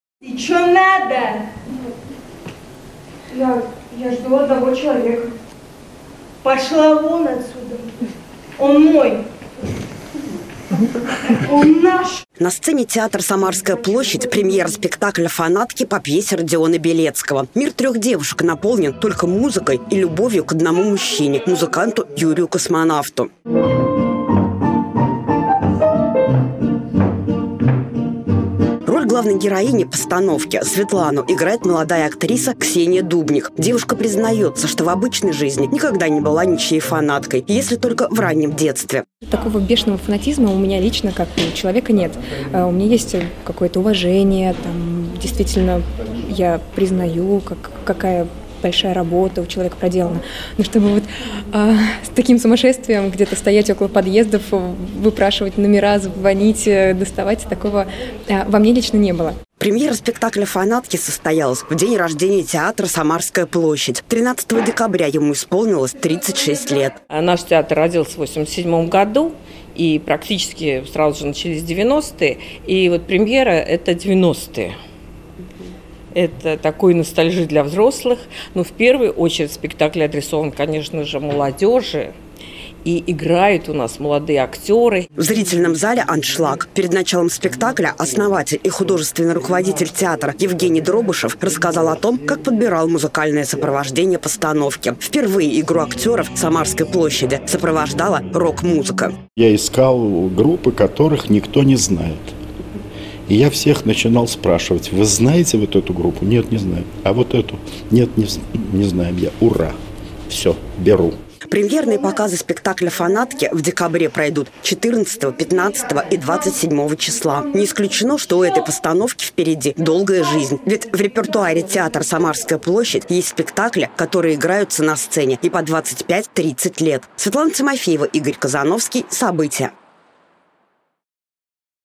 В театре "Самарская площадь" состоялся премьерный показ спектакля "Фанатки"
В муниципальном театре "Самарская площадь" состоялась премьера спектакля "Фанатки" по пьесе Родиона Белецкого. Он прошел в день рождения театра,  36-летие со дня рождения. Из зрительного зала репортаж корреспондента "Событий" Светланы Тимофеевой.